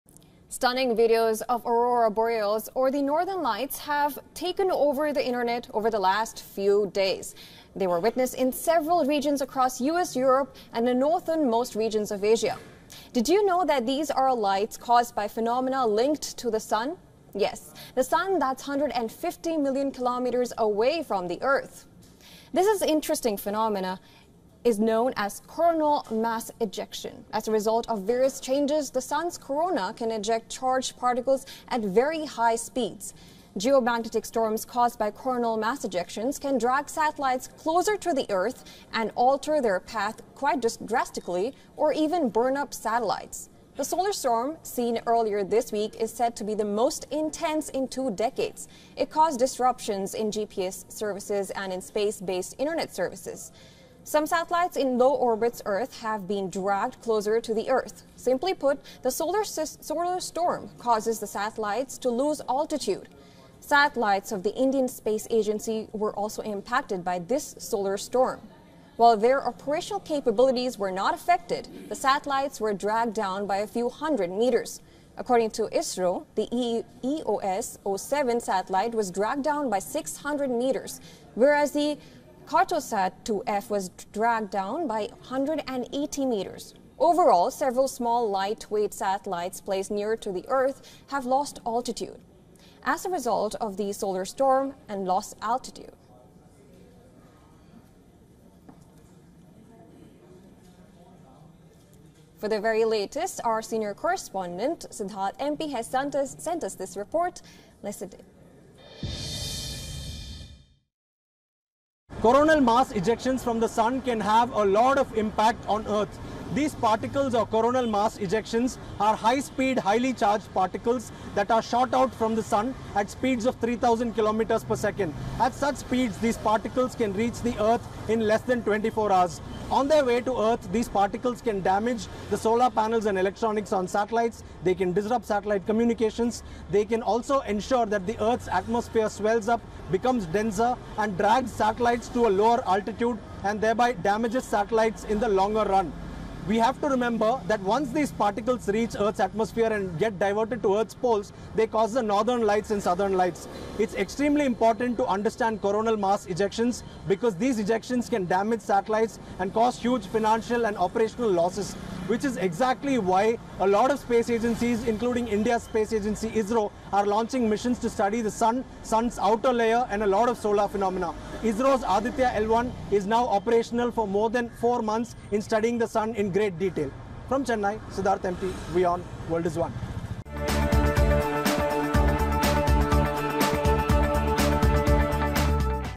Furious Sun unleashes massive solar flare, biggest flare hits Earth in 10 years | Latest News | WION
The Sun's volatile activity peaked late Tuesday when the colossal sunspot AR3664 unleashed a massive solar flare, the most powerful eruption of the current solar cycle. Watch in for more details!